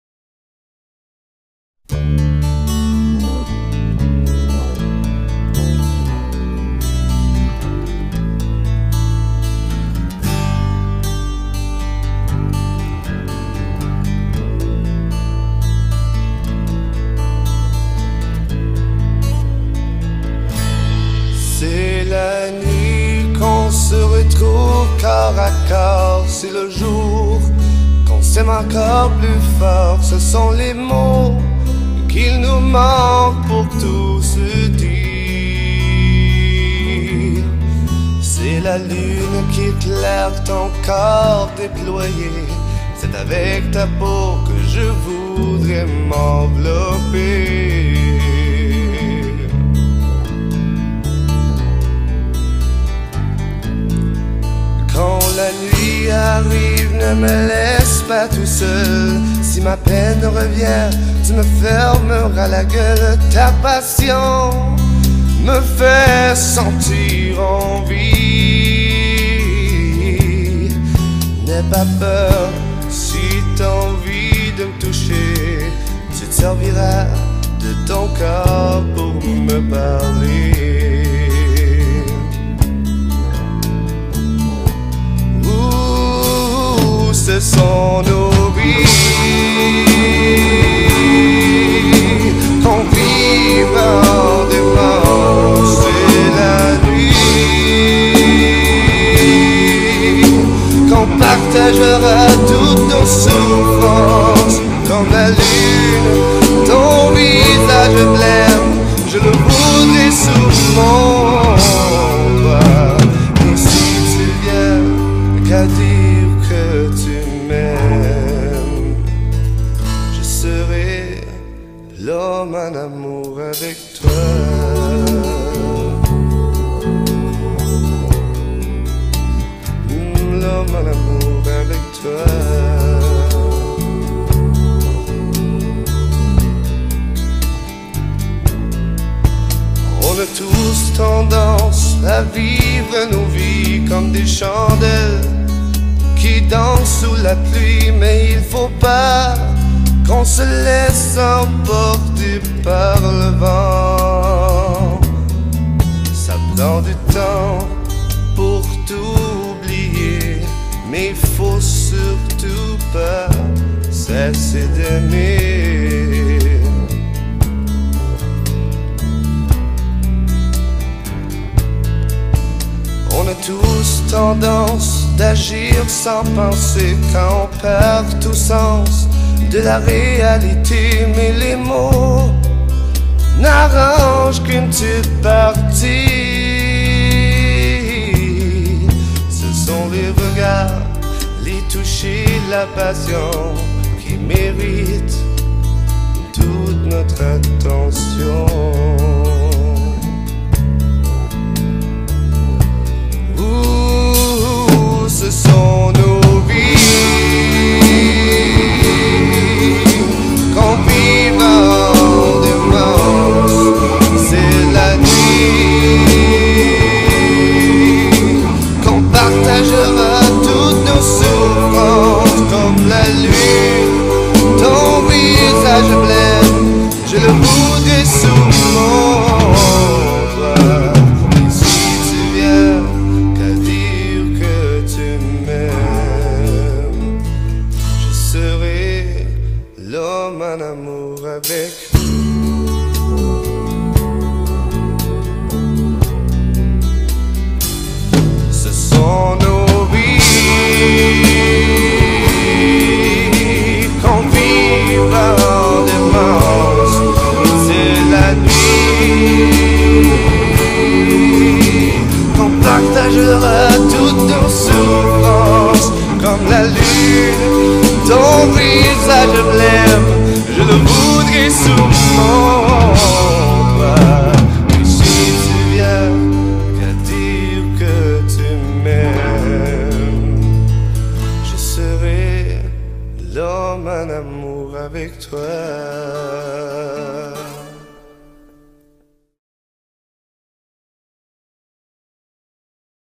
In memory of : Bob Robert Gaudreau
Enjoy... Bob is singing his last composition in this video